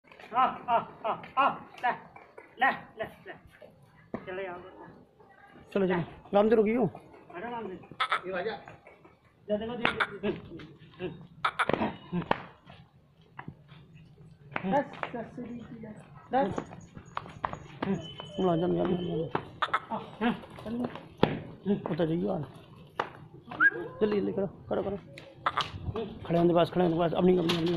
ROCKET KAAN SPOTTED COLOUR BARBARI FEMALE KA LOT FOR SALE, Plz Watch & Subscribe
COLOUR FULL
Barbari female
गाबिन और खाली
Ka lot for sale
*Rocket Kaan*
Red, White, Black
Spotted Body mei